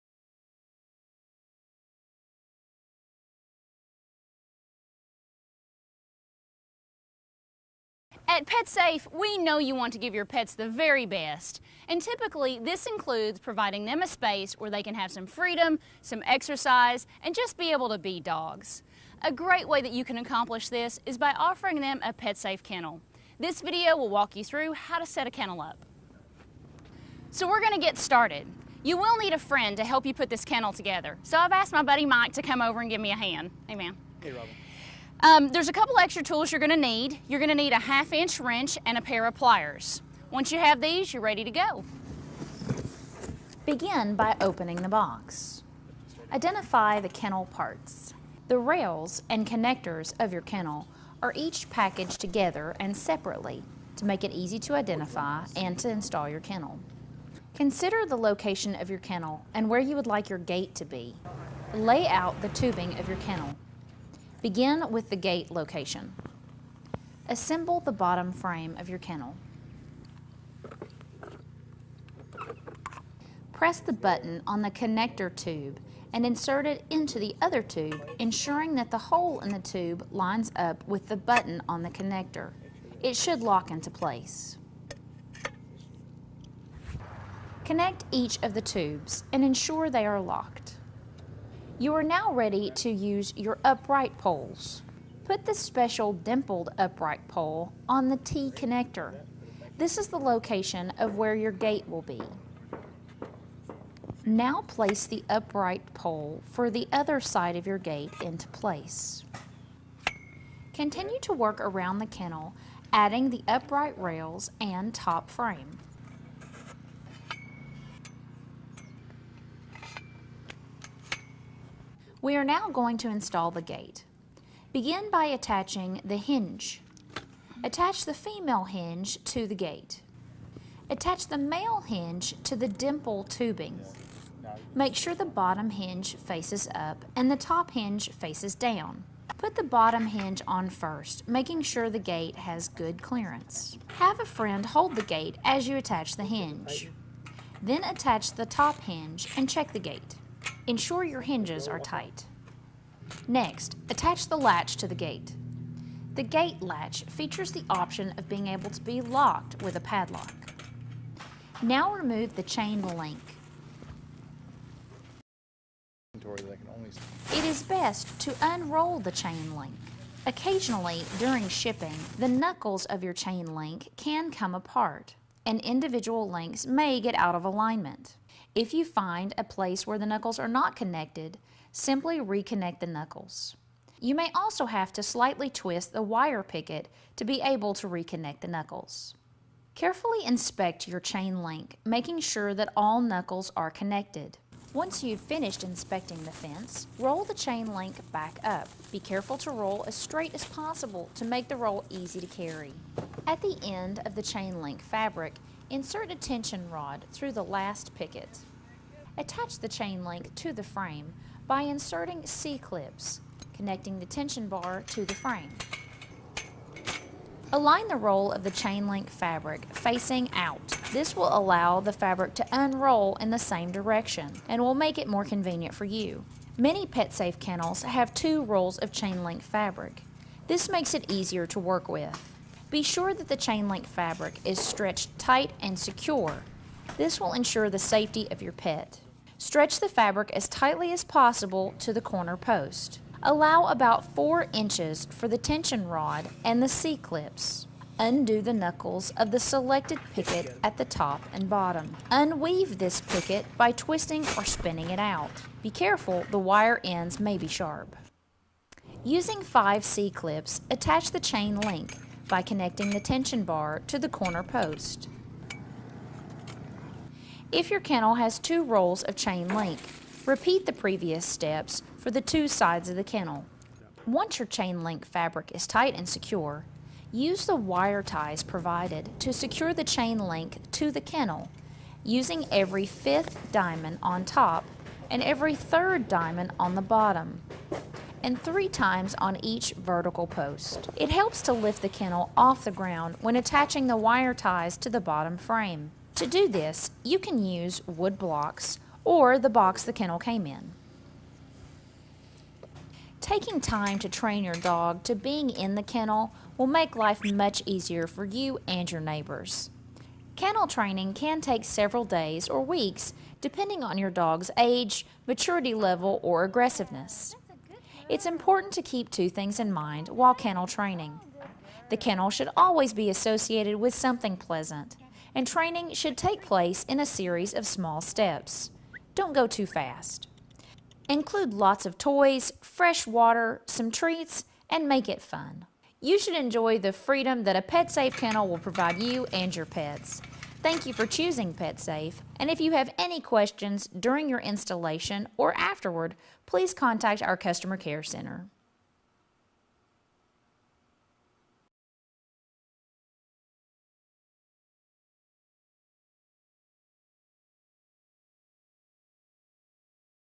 Petsafe Dog Run/Pen Set up (Irish Pet Supplies)
Ireland supplier of Petsafe Dog run..Can place order on our website and see full details.
www irishpetsupplies com
Free next delivery.